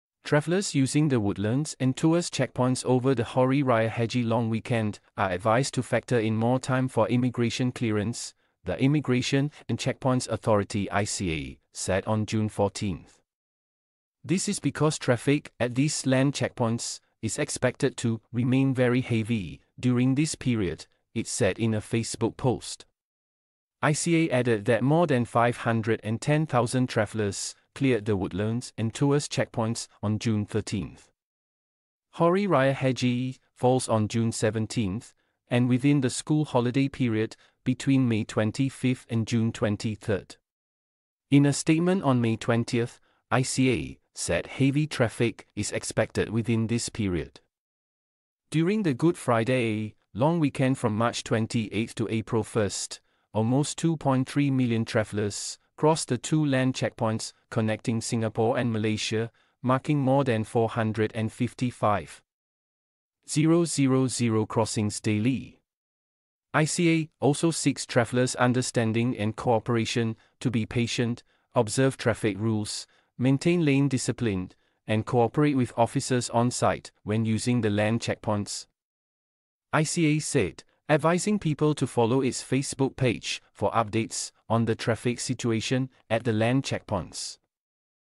Very heavy traffic expected at Woodlands, Tuas checkpoints over Hari Raya Haji weekend: ICA
💗📌 Singapore